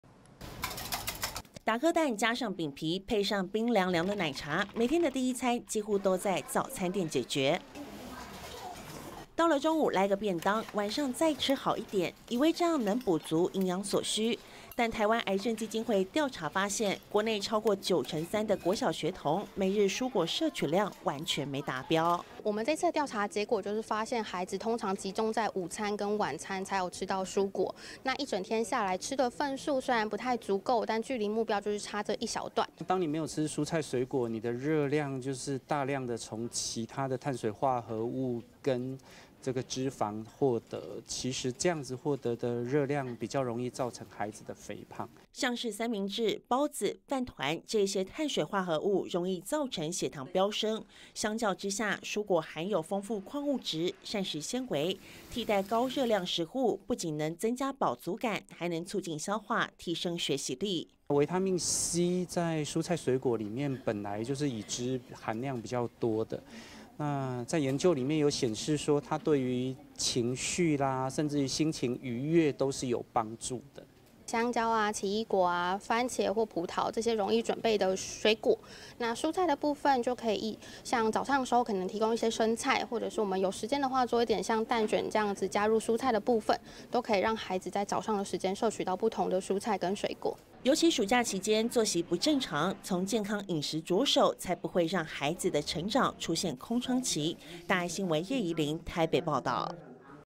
天天5蔬果！九成學童不達標
國健署統計，高達八成國人蔬果攝取不足，以學生比例最高。台灣癌症基金會針對國小孩童進行調查，發現超過九成的學生，無法達到每天五蔬果的攝取，長期營養流失，容易出現免疫力低下，影響發育、睡眠等等問題。營養師表示，相較於午晚餐，孩子在早餐攝取蔬果的比例最低，建議家長可以準備蘋果、葡萄、奇異果等方便取用的水果，補足每日所需的營養。

打顆蛋，加上餅皮，配上冰涼涼的奶茶，每天的第一餐，幾乎都在早餐店解決。到了中午，來個便當，晚上再吃好一點，以為這樣能補足營養所需。但台灣癌症基金會調查發現，國內超過九成三的國小學童，每日蔬果攝取量完全沒達標。

台灣癌症基金會營養師 徐桂婷：「我們這次的調查結果發現，孩子通常集中在午餐跟晚餐，才有吃到蔬果，一整天下來吃的份數，雖然不太足夠，但距離目標就是差這一小段。」

小兒科醫師 陳木榮：「當你沒有吃蔬菜水果，你的熱量就是大量從其他的碳水化合物跟脂肪獲得，其實這樣子獲得的熱量，比較容易造成孩子的肥胖。」

像是三明治、包子、飯糰，這些碳水化合物容易造成血糖飆升。相較之下，蔬果含有豐富礦物質、膳食纖維，替代高熱量食物，不僅能增加飽足感，還能促進消化，提升學習力。

小兒科醫師 陳木榮：「維他命C在蔬菜水果裡面，本來就是已知含量比較多的，在研究裡面有顯示說，它對於情緒，甚至心情愉悅，都是有幫助的。」

台灣癌症基金會營養師 徐桂婷：「香蕉、奇異果、番茄或葡萄，這些容易準備的水果；蔬菜的部分，像早上的時候，可能提供一些生菜，或者是我們有時間的話，做一點像蛋捲這樣子，加入蔬菜的部分，都可以讓孩子在早上的時間攝取到不同的蔬菜跟水果。」

尤其暑假期間作息不正常，從健康飲食著手，才不會讓孩子的成長出現空窗期。

葉宜琳 台北報導